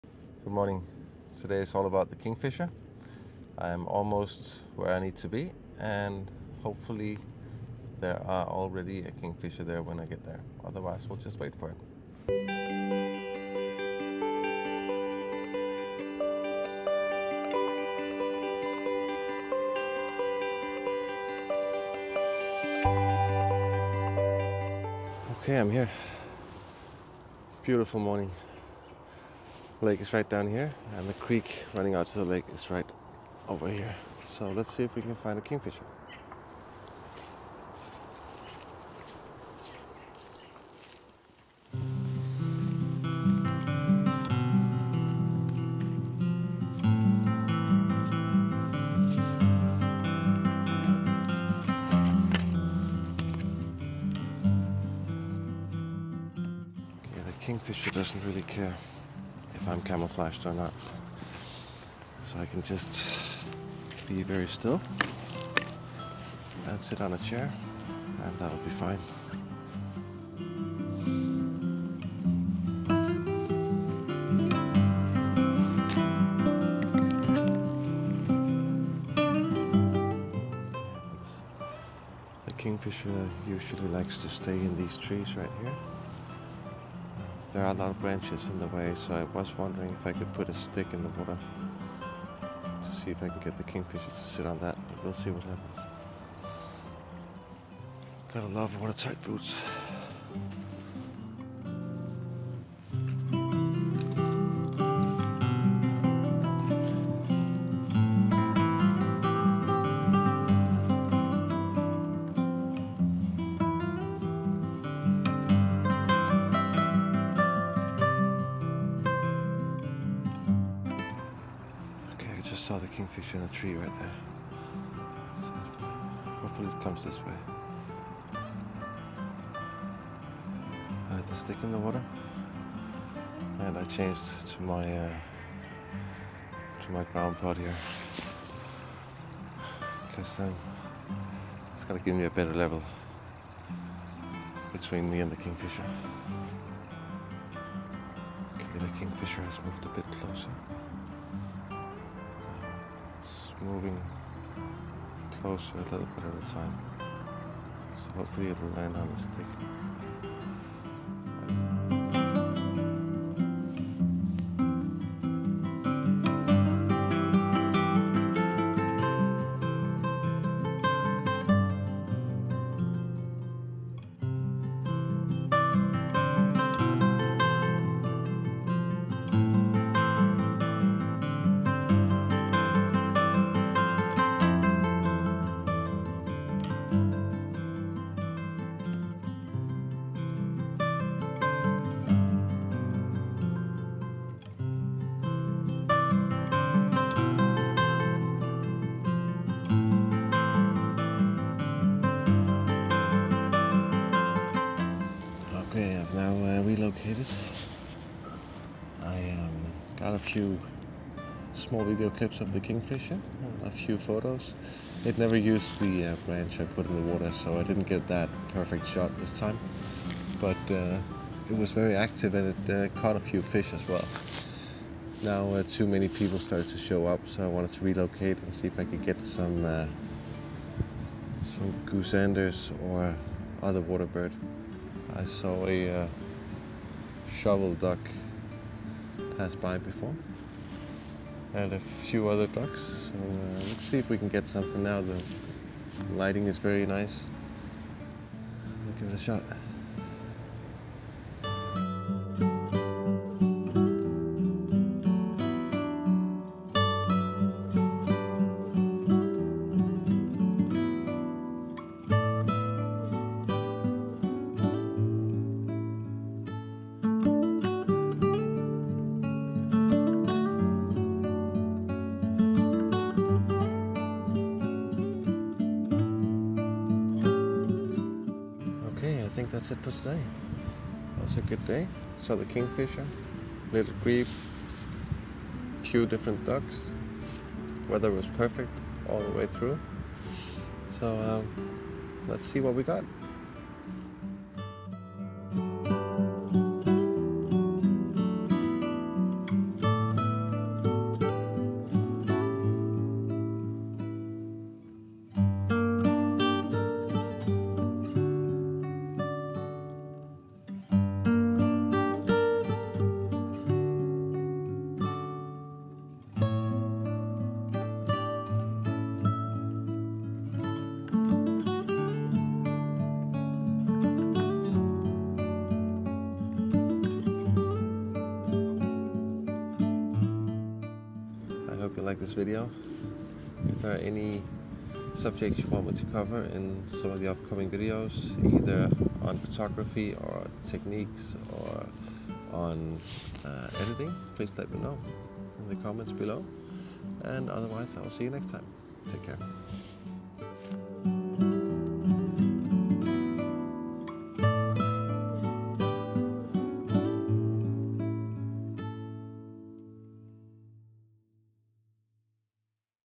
BIRD PHOTOGRAPHY - KINGFISHER and other waterbirds
I’m going to a local lake to photograph kingfisher, shovelers and grebes

To see more of my work and my photographic journey follow me on these platforms for daily stories and images: